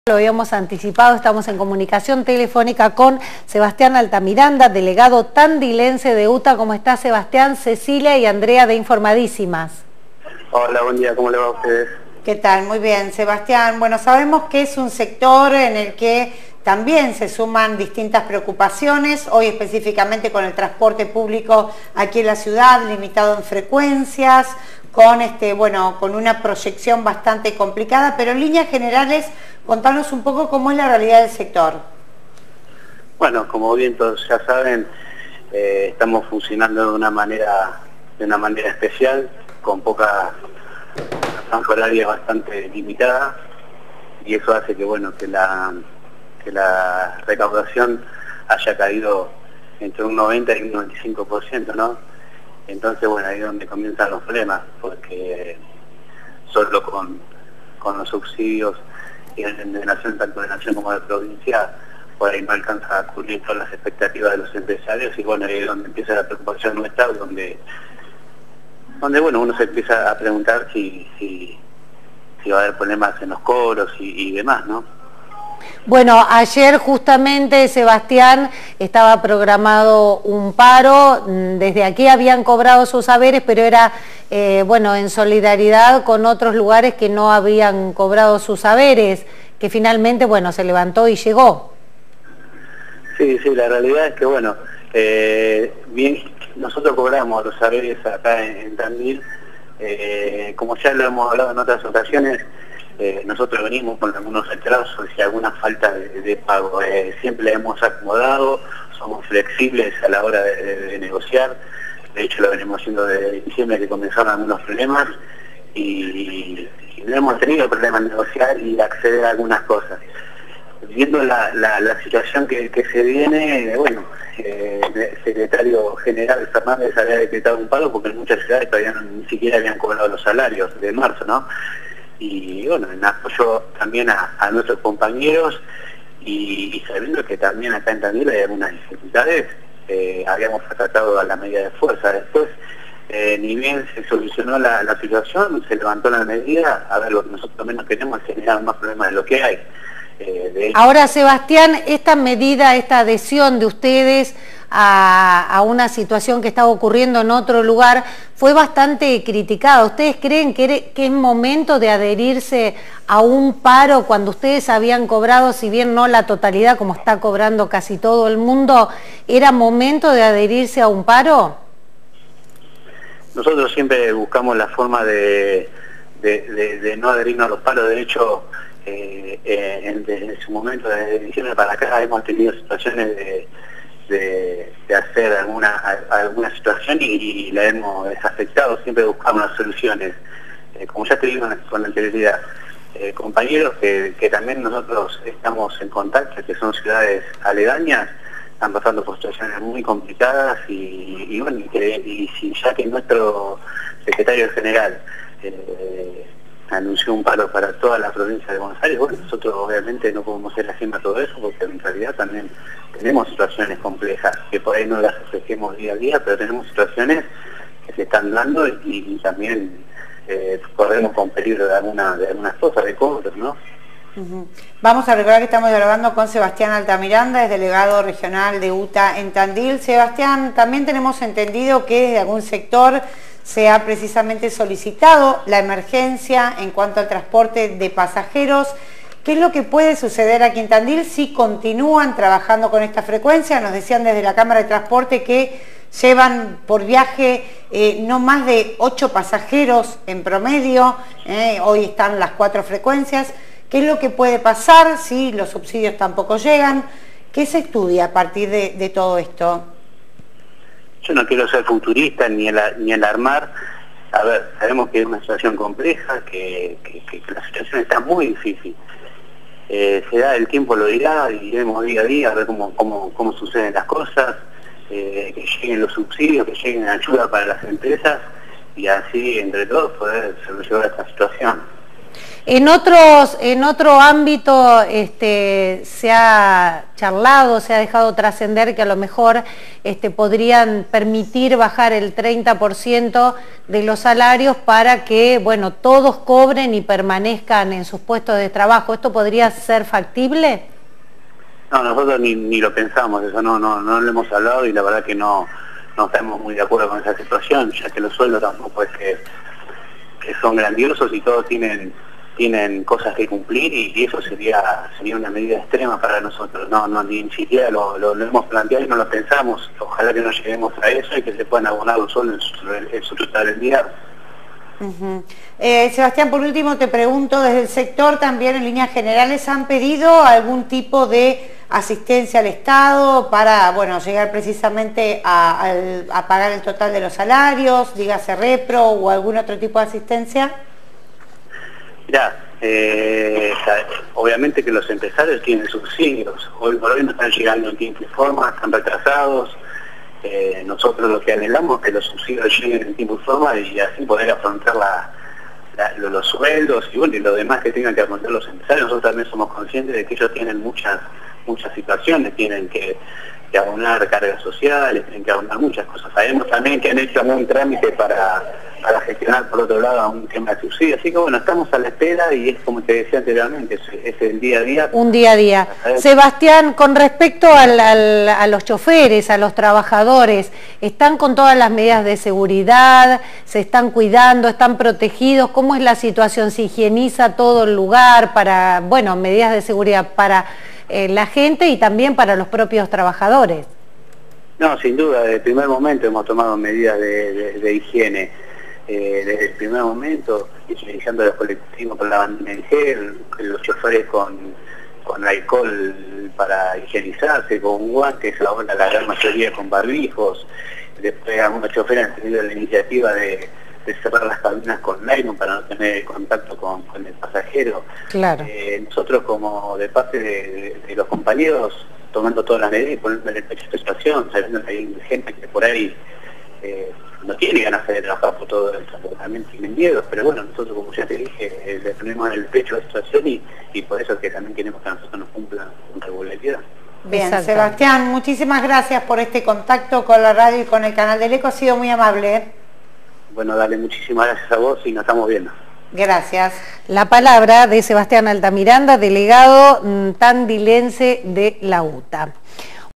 ¿Se estudia una vuelta gradual en los horarios de los recorridos de colectivos?
Sebastian Altamiranda - Delegado Tandilense UTA dialogó con Informadisimas por Eco TV y la 104.1 Tandil FM.